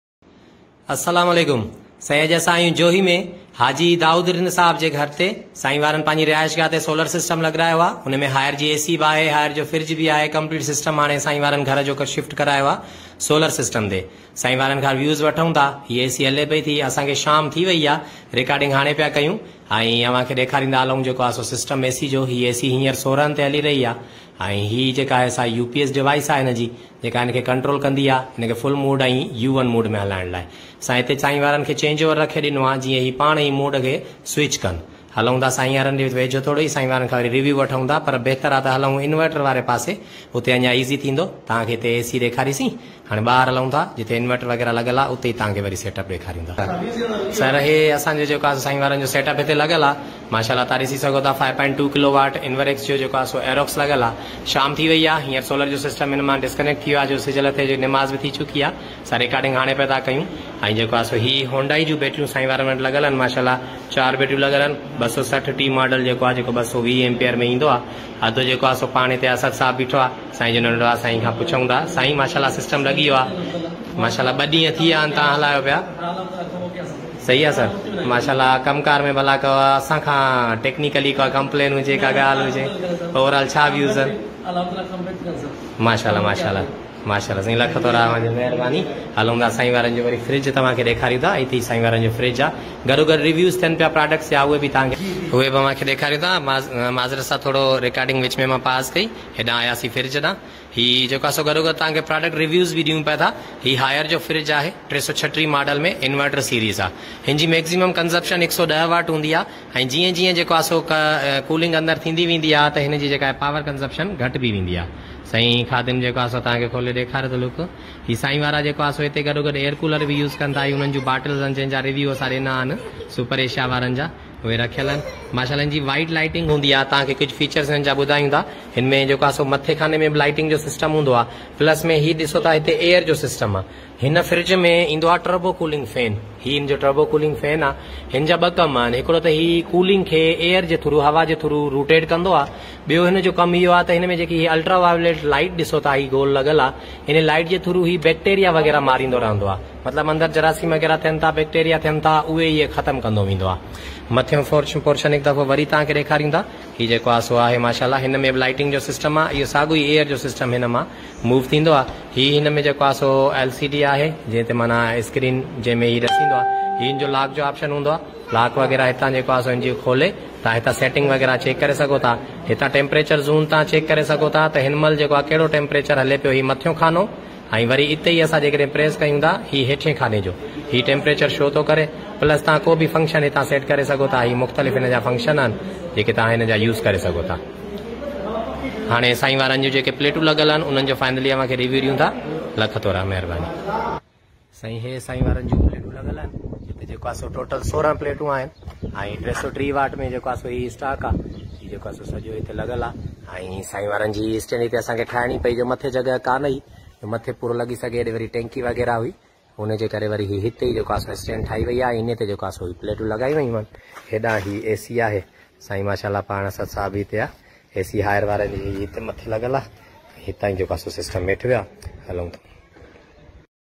Solar System For AC And Home Appliances
الحمد للہ... جوهي۾ ڳوٺ بخش علي رند ۾ سيٺ حاجي دائود رند صاحب جي رهائش گاه تي سولر سسٽم انسٽال ڪيو آهي.
5.2 انوريڪس ايروڪس
هنڊائي ٽيوبلر بيٽريز ڪوريين ٽيڪنالاجي
16 سولر پينل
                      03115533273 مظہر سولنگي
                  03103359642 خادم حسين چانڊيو